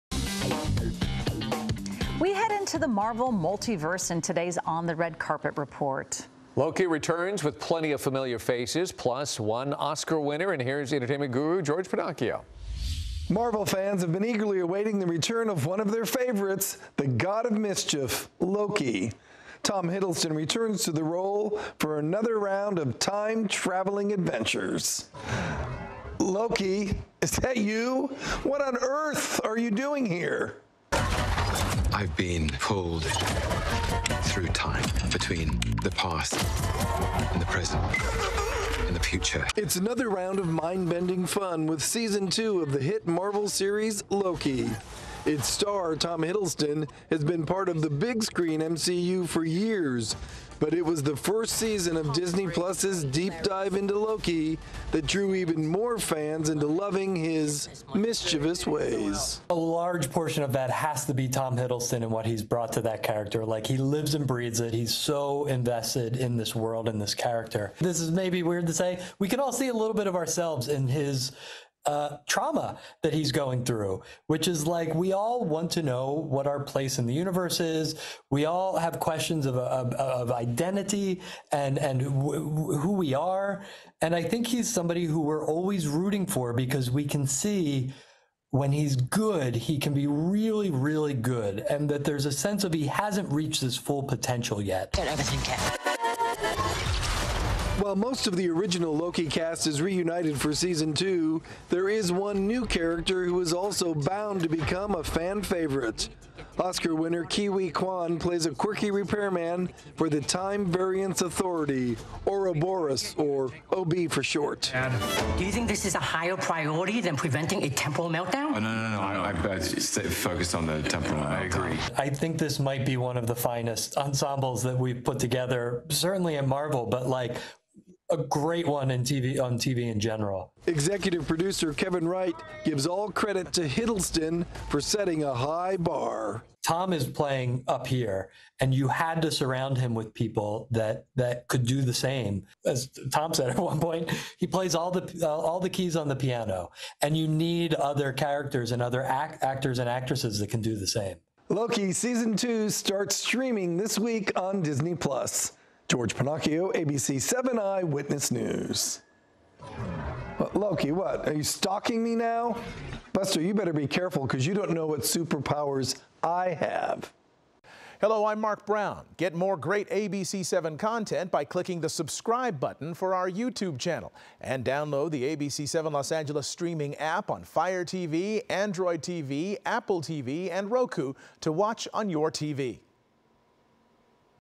Loki returns for season 2 of time-bending adventures, with new cast additions
The next season of the time-slipping, mind-bending series "Loki" with Tom Hiddleston is out this week on Disney+.